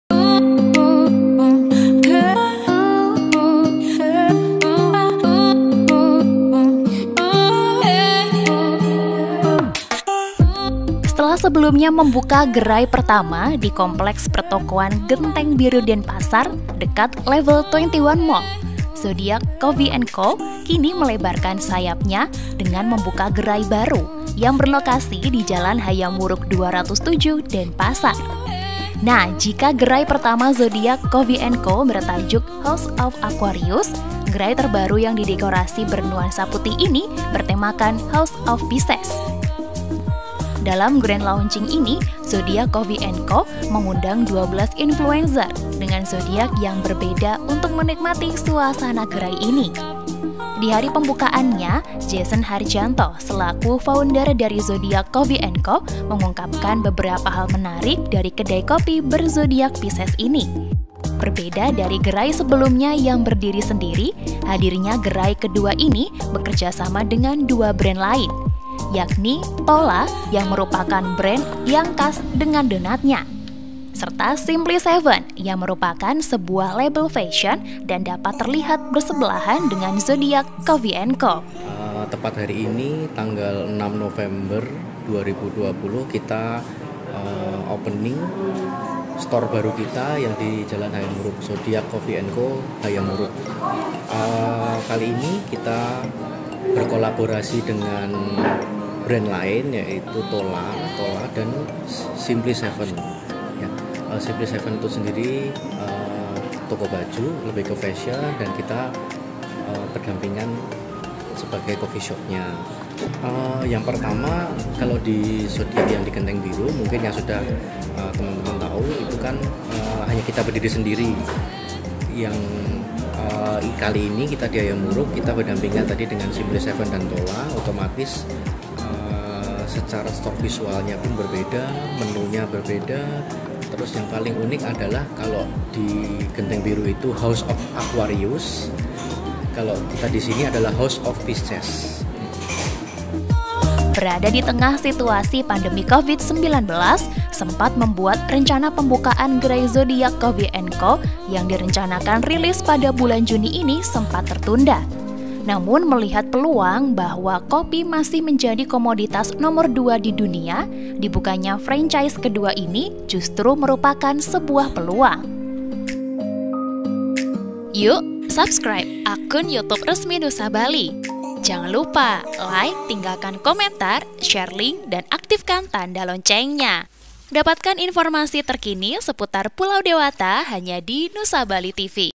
House of Pisces, Gerai Kedua Zodiac Coffee & Co di Buka di Hayam Wuruk Denpasar
Zodiac Coffee & Co kini melebarkan sayapnya dengan membuka gerai baru yang berlokasi di Jalan Hayam Wuruk 207 Denpasar. Nah, jika gerai pertama Zodiac Coffee & Co bertajuk House of Aquarius, maka gerai terbaru yang didekorasi bernuansa putih ini bertemakan House of Pisces.

Reporter : Yulia
Videographer : Arief Wanso